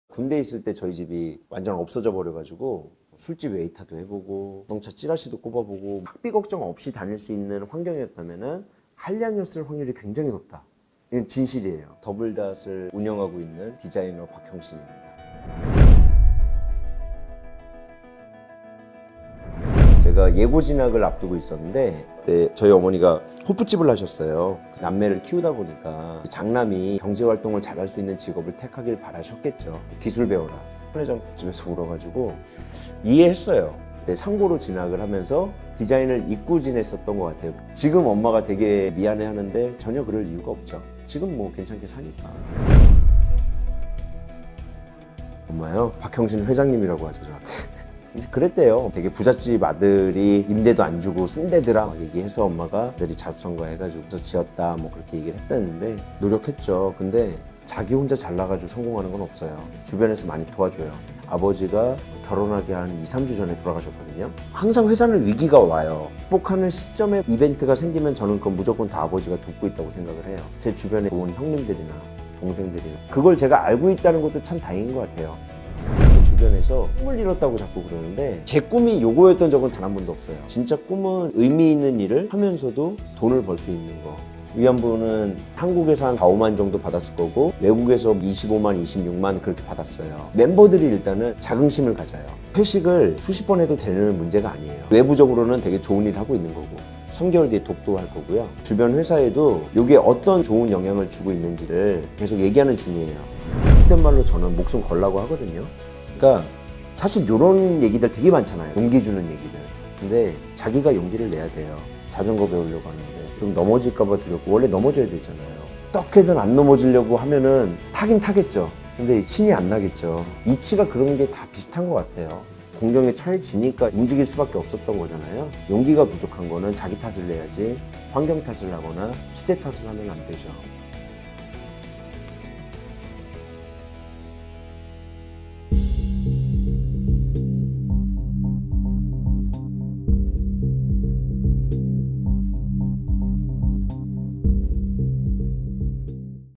더블닷 대표 박형신 / 상상속의 건물을 마침내 짓다
혼자 잘나서는 성공할 수 없다.
세상에는 좋은 영향을 주변 사람에겐 도움을 나누는
디자인 에이전시 더블닷 대표 박형신의 이야기.